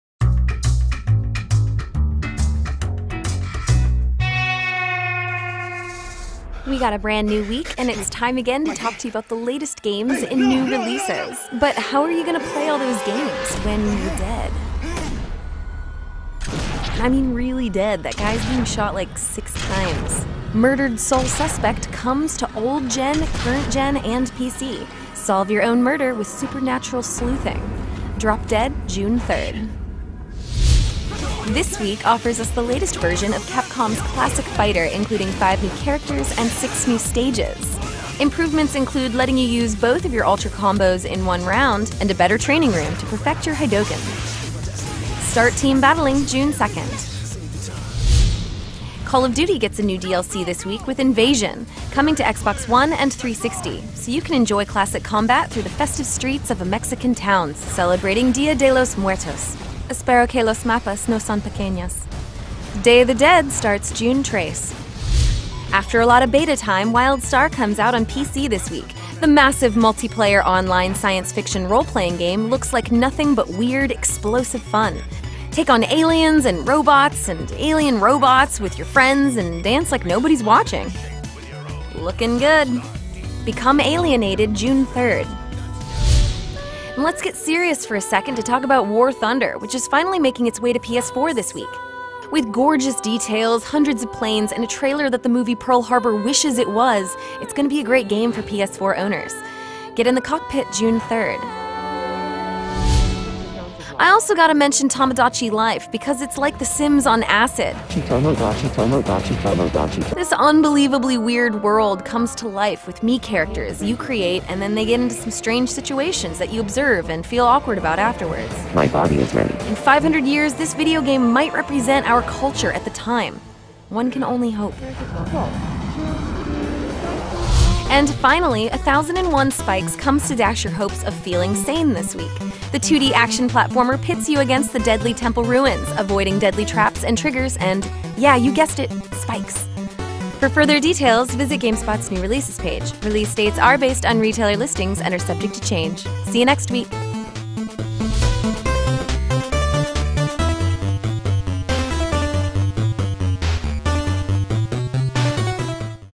Wildstar, Murdered Soul Suspect, COD DLC - New Releases
The week before E3 gives us Wildstar, Murdered: Soul Suspect, Call of Duty: Ghosts Invasion DLC, Ultra Street Fighter IV, Tomodachi Life, 1001 Spikes and War Thunder.